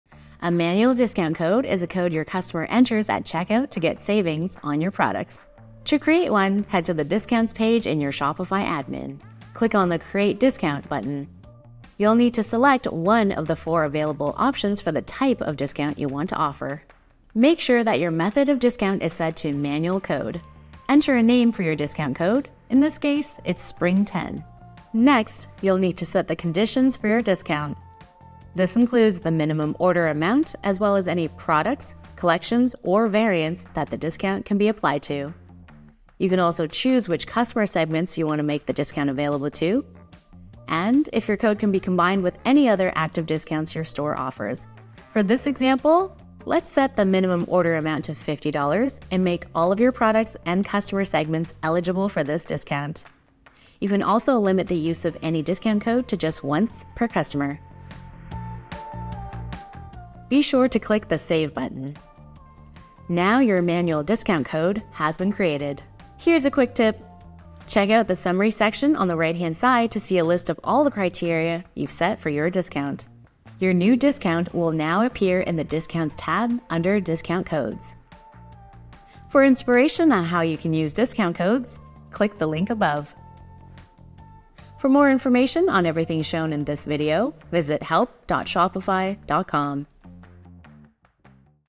How to Setup Manual Discount Codes || Shopify Help Center
Learn the difference between manual and automatic discount codes. Walk through how to setup manual discount codes on your e-commerce store.